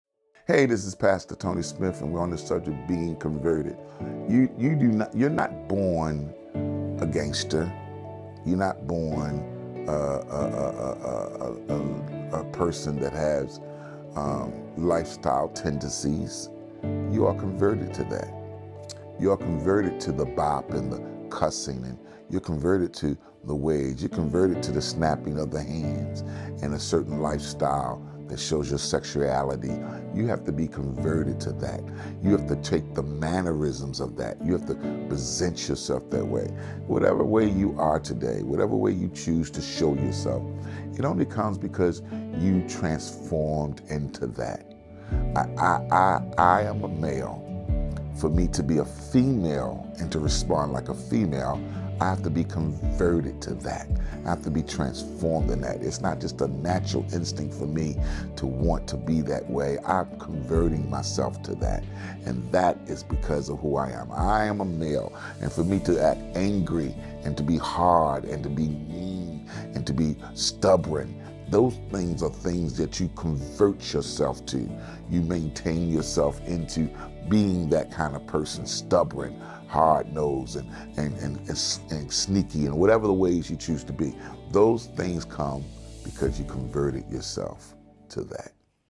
Daily Dose of Victory | Topic: Being Converted | Your Mannerisms | Wednesday April 24, 2024
Tune in to hear a "Daily Dose of Victory" each and every weekday at 10am.

Topic: Being Converted | Your Mannerisms

Be sure to share and encourage your friends and followers with these weekly Daily Devotions with vmipts